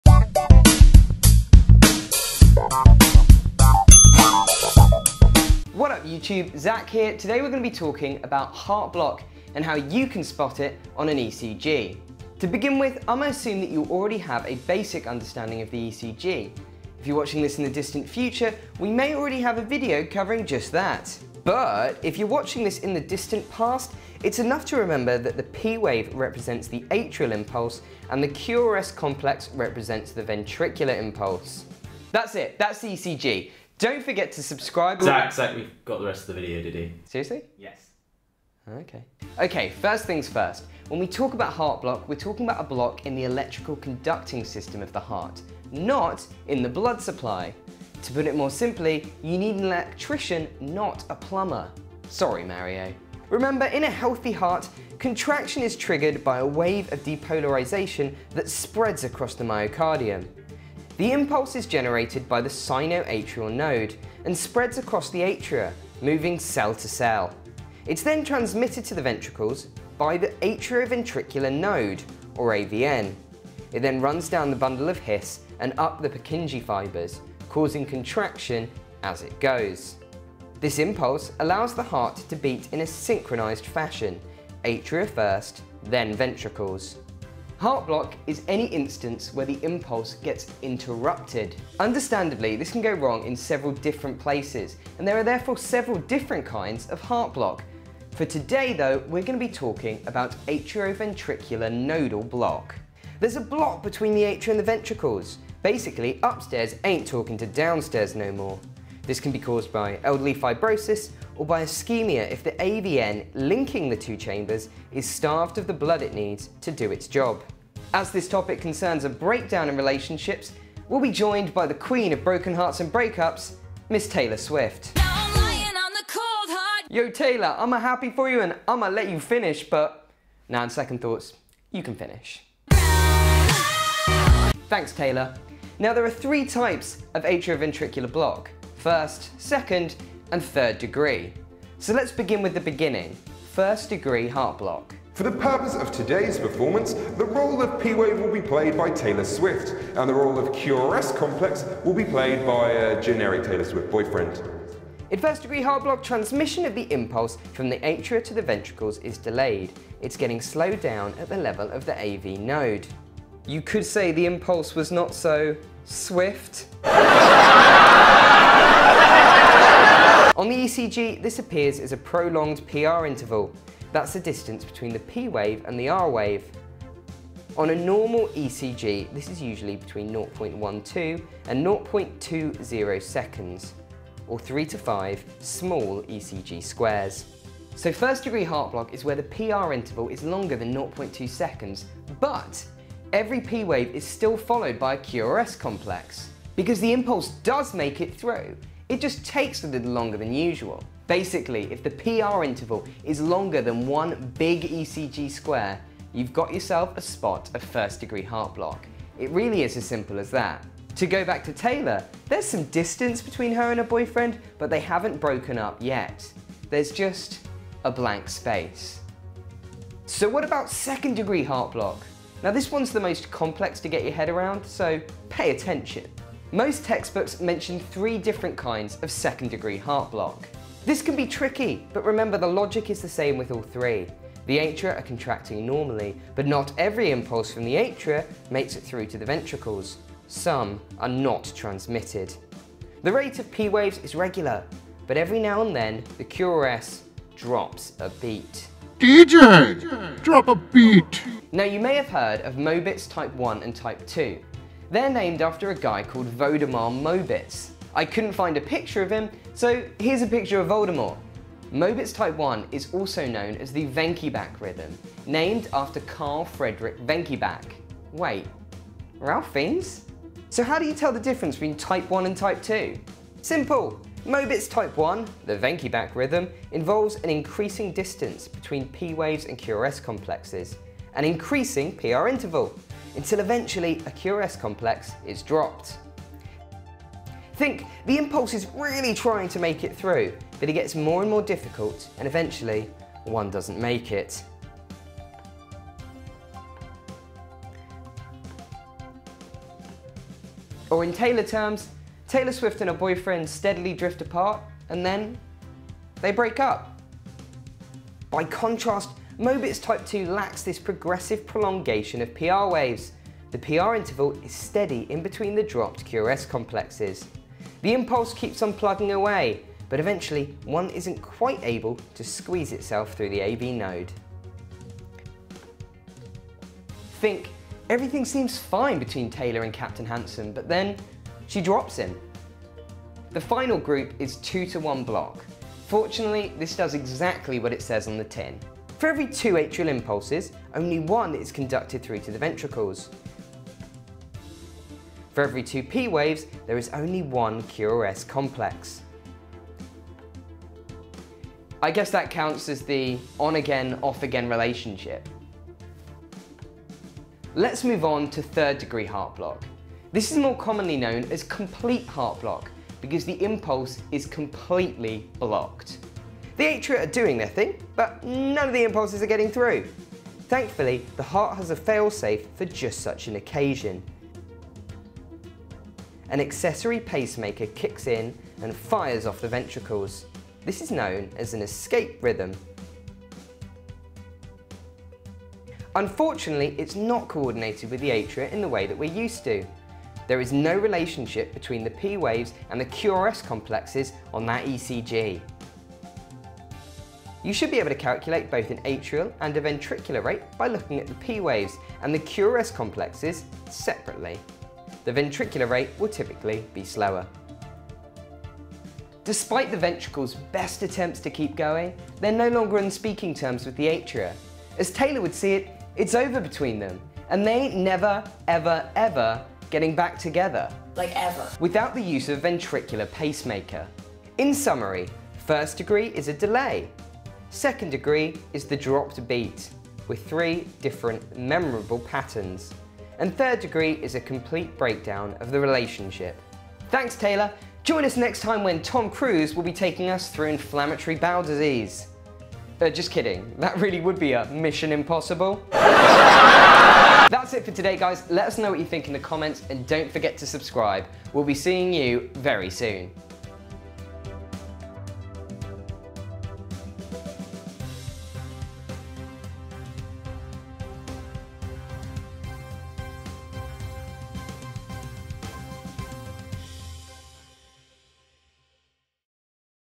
Heart Block Made Easy (feat Taylor Swift)
Struggling with heart block? Look no further – today we cover how to identify atrioventricular block on an ECG with the help of pop superstar Taylor Swift. Who better to teach us about broken hearts and break ups?

Heart block can be divided into three types – first, second and third degree.

First degree heart block is where the electrical impulse is delayed at the level of the AV node, leading to a longer period between atrial systole and ventricular systole. This manifests on the ECG as a prolonged PR interval – the time between the p wave and QRS complex.

A normal PR interval is between 0.12 and 0.20 seconds – or three to five small ECG squares. If there is more than one large square between the p wave and the QRS complex, first degree heart block is present.

Second degree heart block is where some of the impulses don’t make it through the AV node – not every p wave is followed by a QRS complex. It can be separated into Mobitz Type I and Type II, and 2:1 heart block. Mobitz Type I is also known as the Wenckebach rhythm.

Mobitz Type I is where there is progressive lengthening of the PR interval, before a QRS complex is dropped. By contrast, the PR interval in Mobitz Type II is stable in between the dropped beats. In 2:1 heart block, a QRS complex appears after every other p wave – only half of the impulses are being conducted.

In complete heart block, none of the impulses are making it through. However, an accessory pacemaker fires off the ventricles – this is known as an escape rhythm. The escape rhythm isn’t co-ordinated with the sinus rhythm however – there is no relationship between p waves and QRS complexes on the ECG. The ventricular rate is typically much slower than the atrial rate.

Spotting heart block is difficult, but will improve with practice. The best way to learn is to look at real life ECGs.

REFERENCES:
1. Longmore M, Wilkinson IB, Davidson EH Foulkes A, Mafi AR (2010). Cardiovascular medicine. In: Oxford Handbook of Clinical Medicine (Eight Edition). Oxford Press. pp 86 – 153
2. Ballinger A (2012). Cardiovascular disease. In: Essentials of Kumar & Clark's Clinical Medicine (Fifth Edition). Saunders Elsevier. pp 407 – 504
3. Davies JER, Nijjer S (2014). Cardiology. In: Kalra, PA. Essential Revision Notes for MRCP (Fourth Edition). PasTest. pp 1 - 64